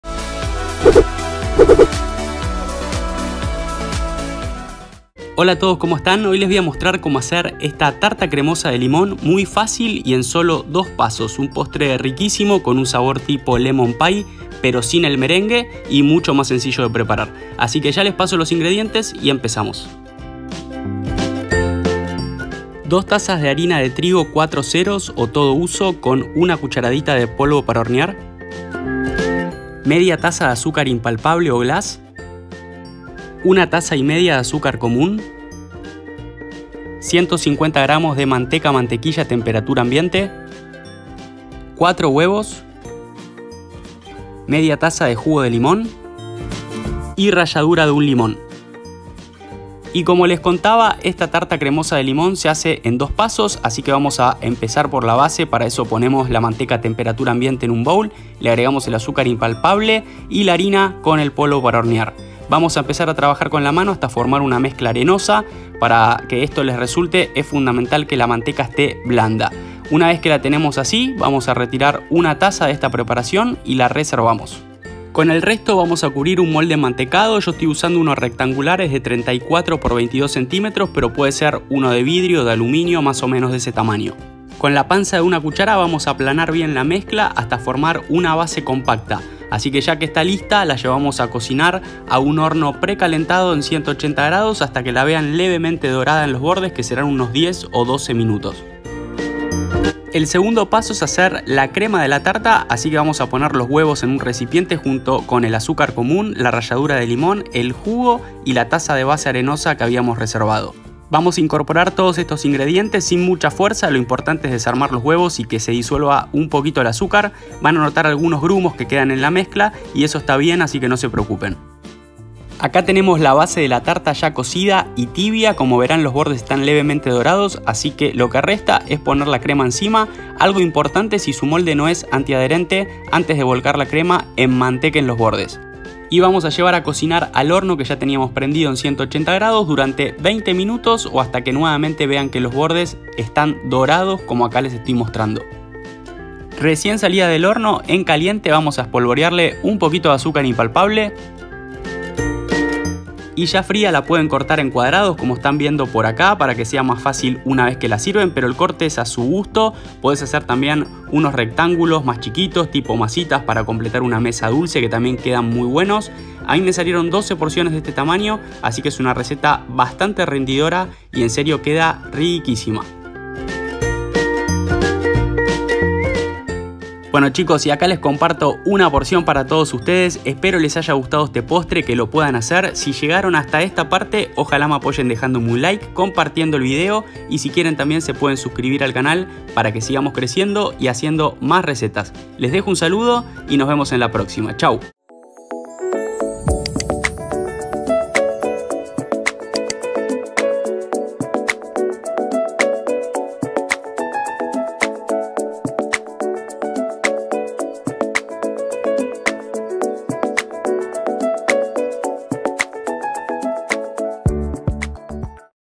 Tarta Cremosa de Limón Fácil en 2 Pasos / MONO 1981
Hola! en este video les muestro cómo hacer una tarta cremosa de limón muy fácil y en sólo dos pasos. Un postre riquísimo, con un sabor tipo lemon pie pero sin el merengue y mucho más sencillo de preparar!

Ingredientes

Base

2 tazas de harina (300 gr) de trigo 0000 o todo uso con una cdta de polvo para hornear

1/2 taza de azúcar impalpable (80 gr)

150 gr de manteca - mantequilla a temperatura ambiente

Crema

1 y 1/2 taza de azúcar común (350 gr)

1 taza de la mezcla de la base

4 huevos

1/2 taza (120 ml)  de jugo de limón